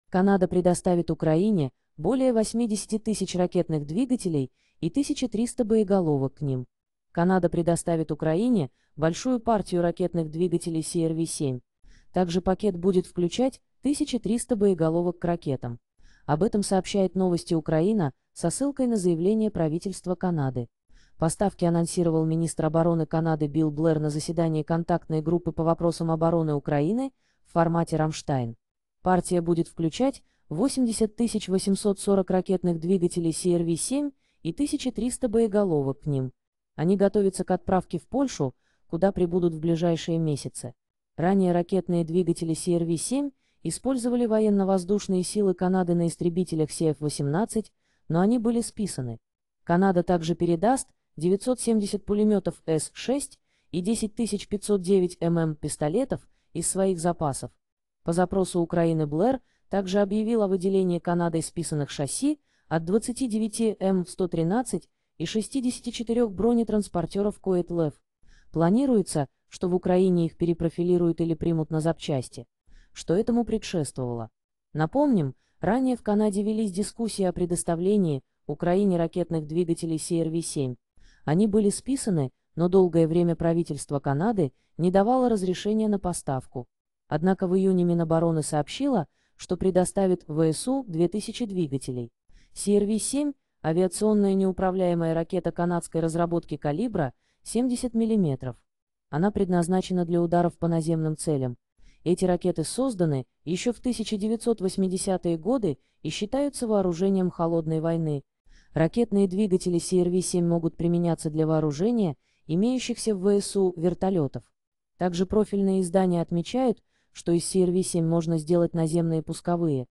Канада предоставит Украине более 80 тысяч ракетных двигателей и 1300 боеголовок к ним.
Канада Военнаяпомощь Ракеты Канада предоставит Украине большую партию ракетных двигателей CRV-7. Также пакет будет включать 1300 боеголовок к ракетам. Об этом сообщает Новости Украина со ссылкой на заявление правительства Канады. Поставки анонсировал министр обороны Канады Билл Блэр на заседании Контактной группы по вопросам обороны Украины в формате "Рамштайн". Партия будет включать 80 840 ракетных двигателей CRV-7 и 1300 боеголовок к ним. Они готовятся к отправке в Польшу, куда прибудут в ближайшие месяцы. Ранее ракетные дви